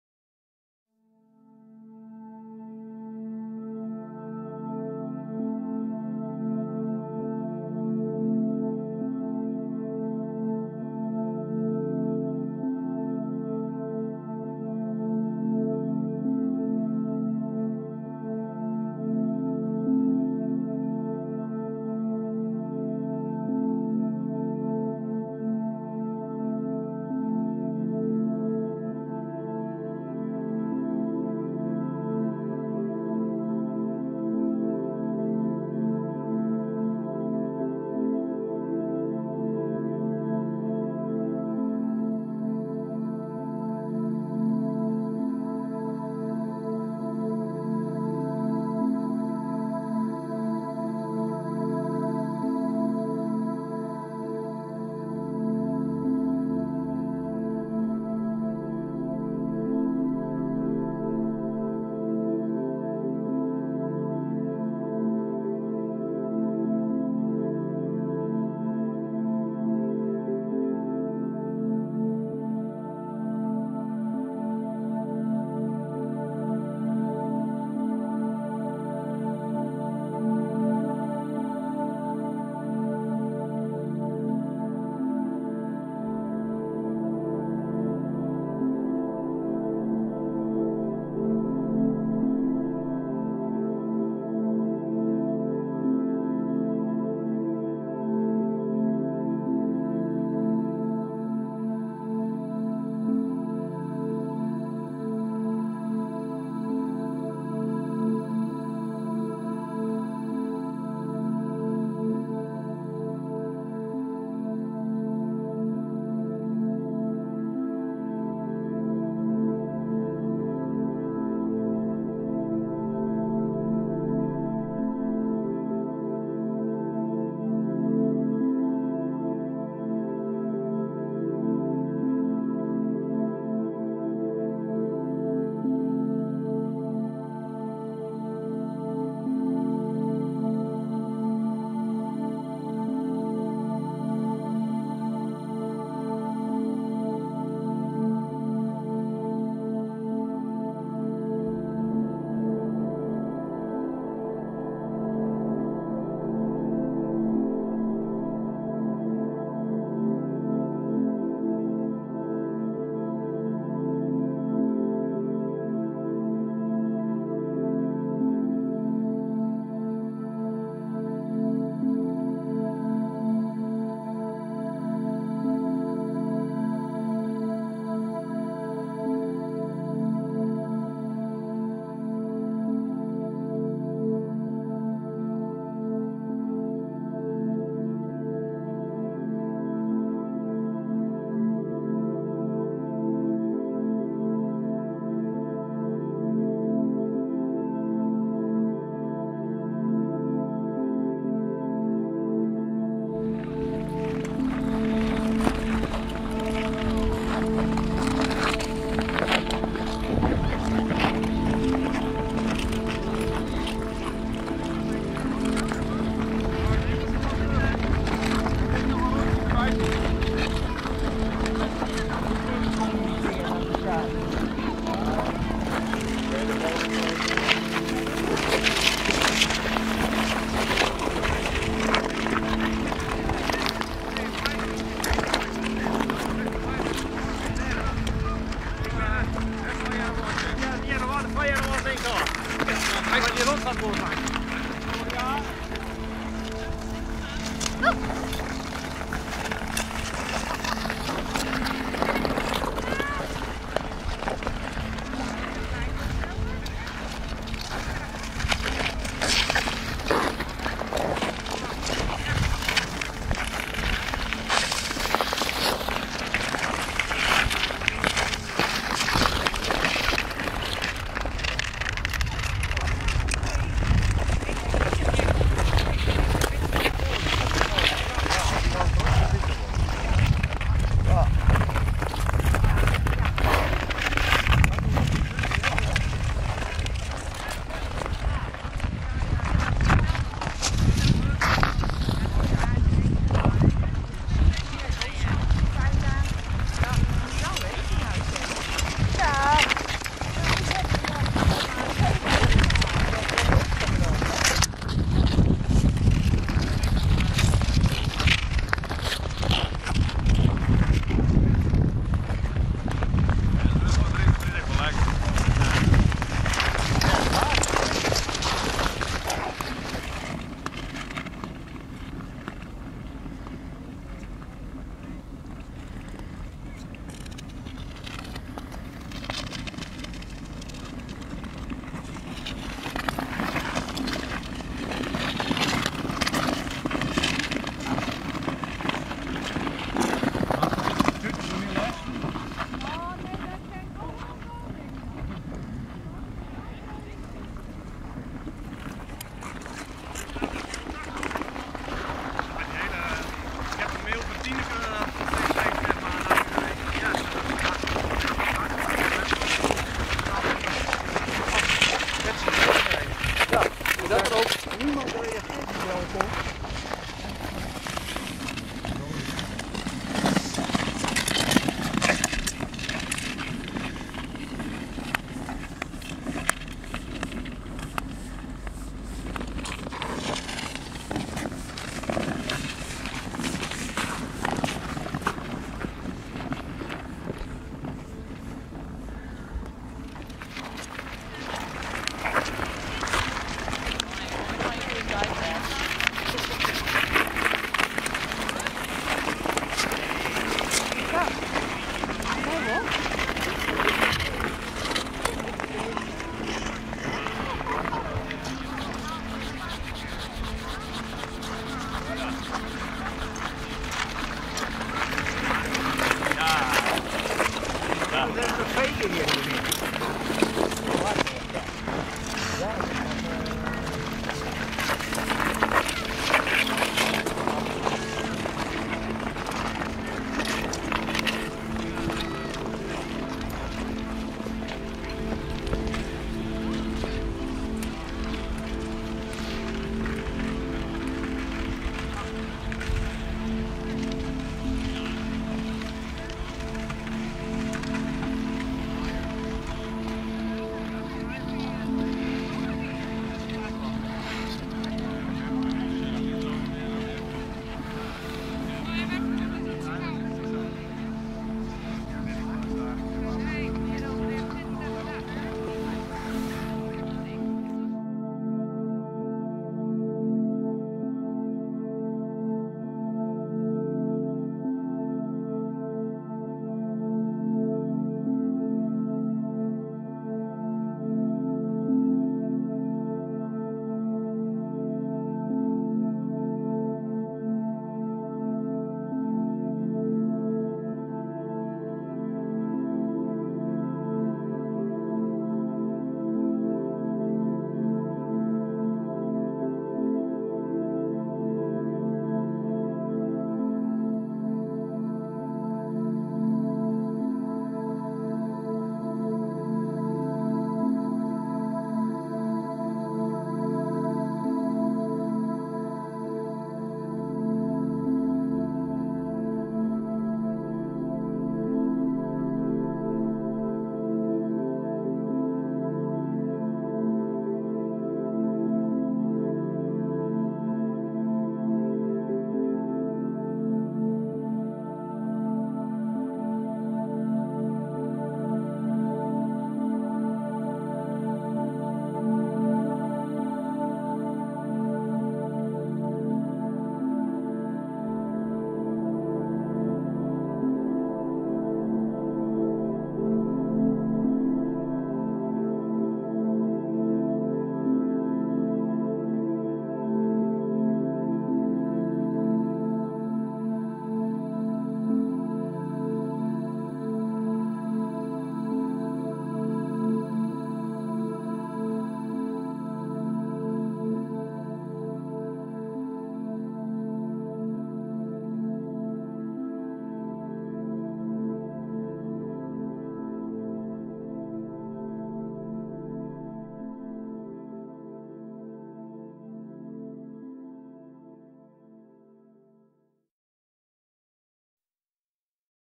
De lange witte winter - deel 10
In de 17-delige serie "De lange witte winter" wordt m.b.v. foto- en videomateriaal een beeld geschetst van de sneeuwrijke winter van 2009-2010 in Fryslân. Ieder filmpje duurt ongeveer 10 minuten ...

Deel 10 is getiteld "IJspret" en begint met een voor Nederland bijzonder verschijnsel, dat zich alleen bij strenge vorst voordoet: poolsneeuw. Daarna gaan we door het besneeuwde en berijpte landschap op weg naar de Jan Durkspolder tussen Oudega en Earnewâld. Begin januari 2010 kon daar net als nu volop worden geschaatst op het ondergelopen land. Het belangrijkste verschil met nu is, dat er toen eerst een baan geveegd moest worden, terwijl we hier deze week nog steeds geen vlokje sneeuw hebben gehad, maar dat zou vandaag (zondag 20 januari 2013) wel eens kunnen veranderen ...